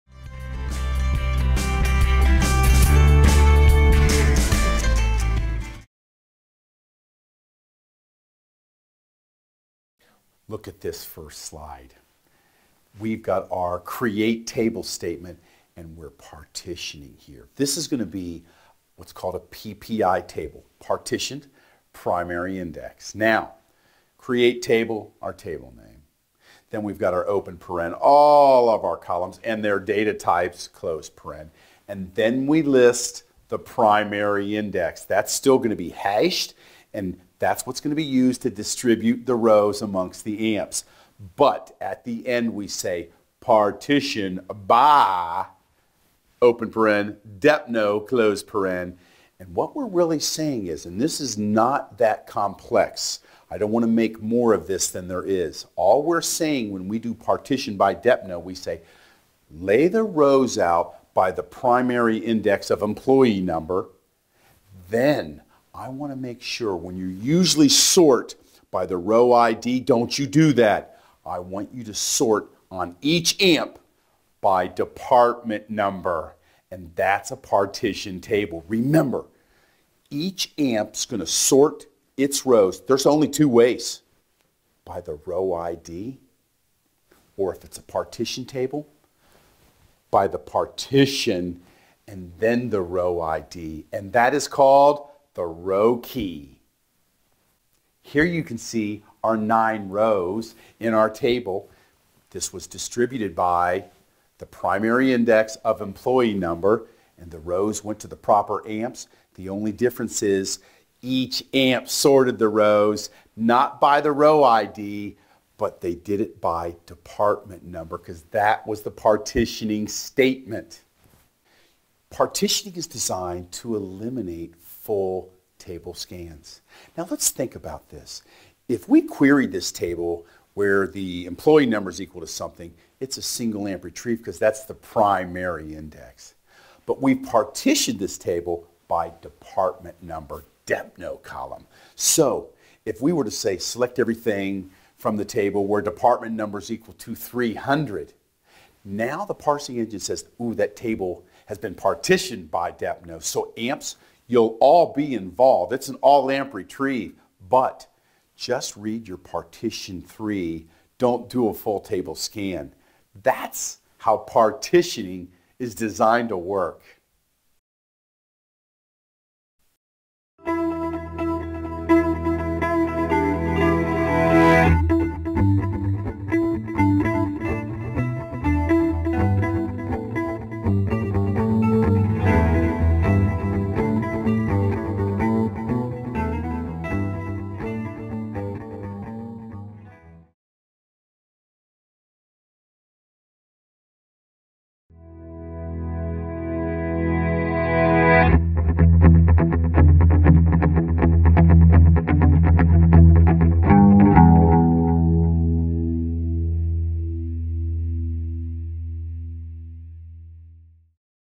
Let's Learn Teradata Lesson 25: Introduction to Simple Partitioning
Explore Teradata with Tera-Tom of Coffing Data Warehousing! In this lesson, learn about Simple Partitioning!